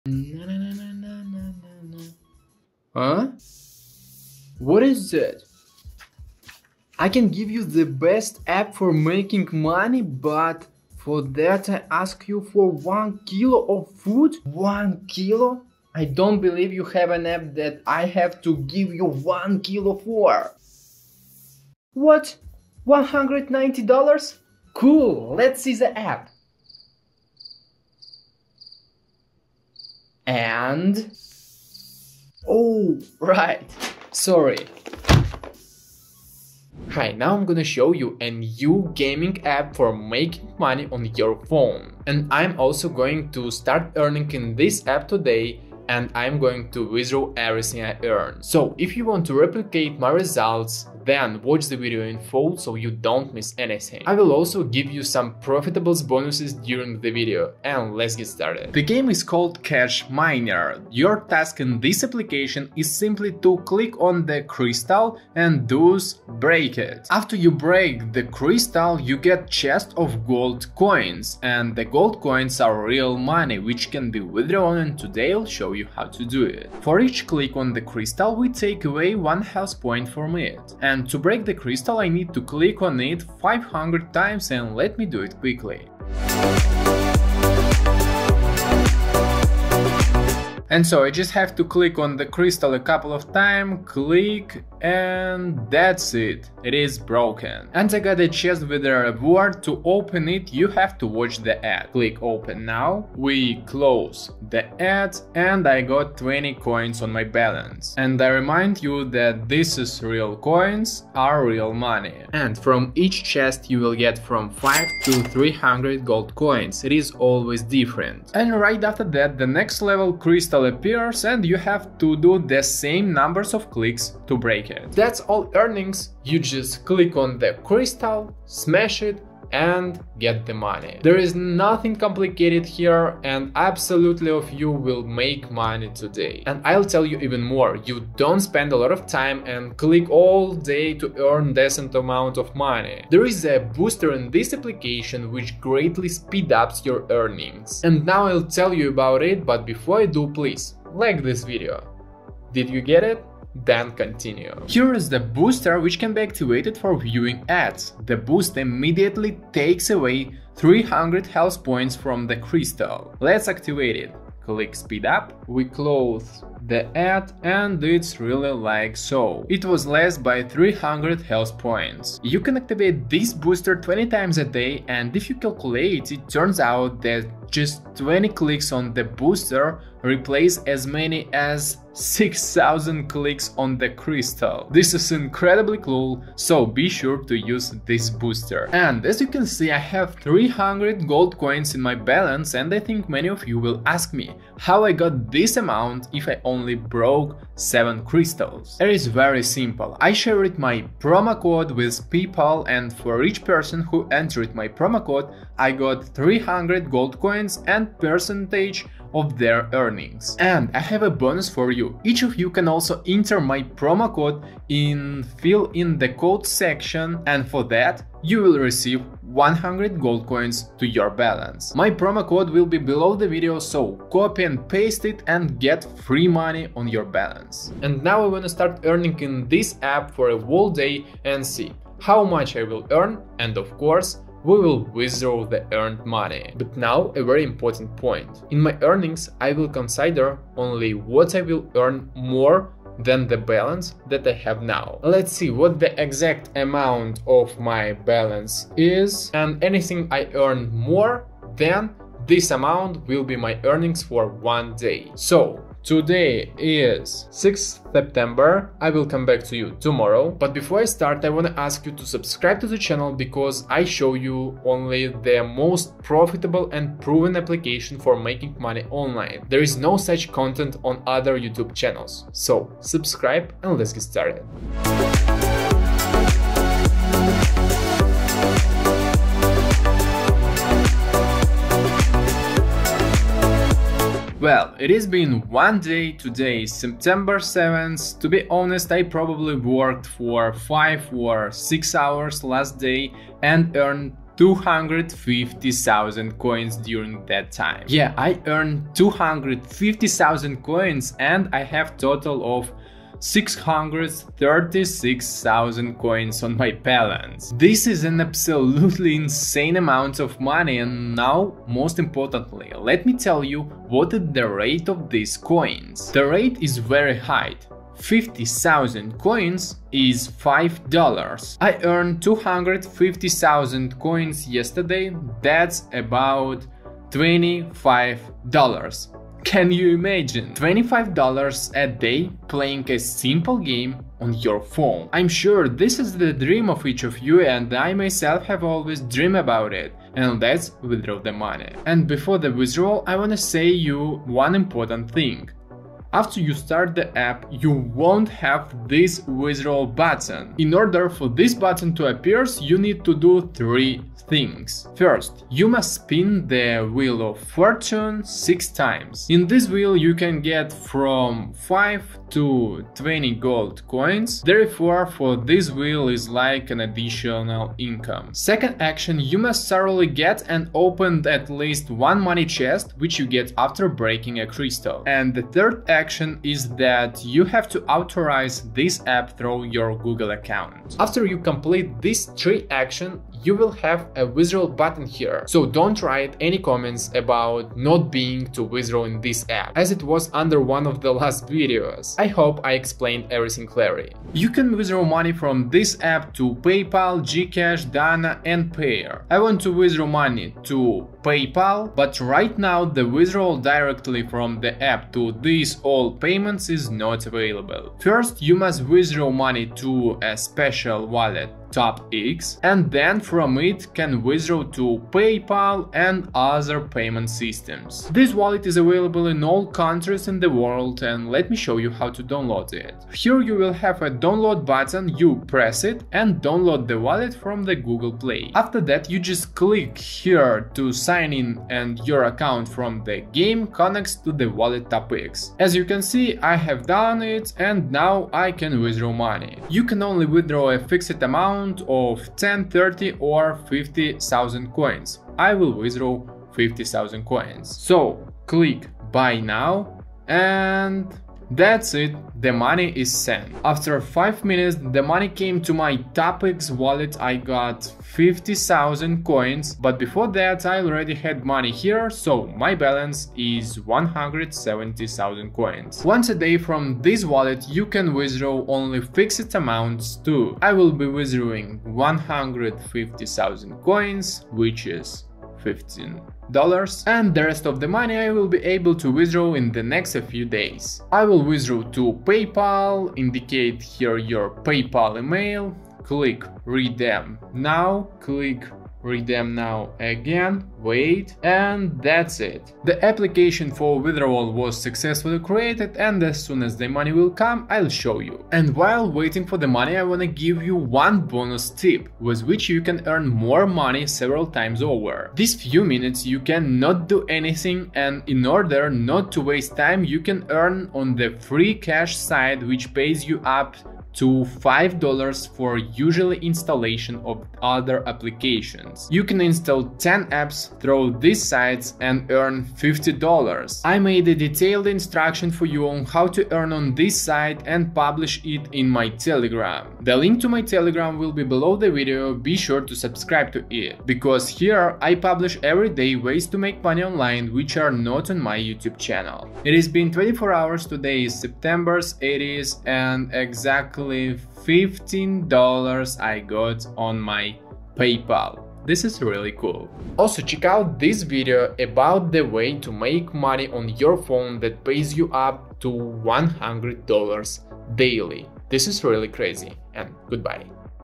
Play Game & Earn $40 Per Hour - (Make PayPal Money Online For Free)
💥 How to earn money online by playing game? Where you can find the best way for making money online that pays you $40 per every hour for playing game? I tried to answer these questions in this video!

💸 The app I used in this video is in pinned comment!
⚡ EARN up to $50 DAILY with the method from my TG

✅ THE BEST WAYS TO MAKE MONEY ONLINE FROM HOME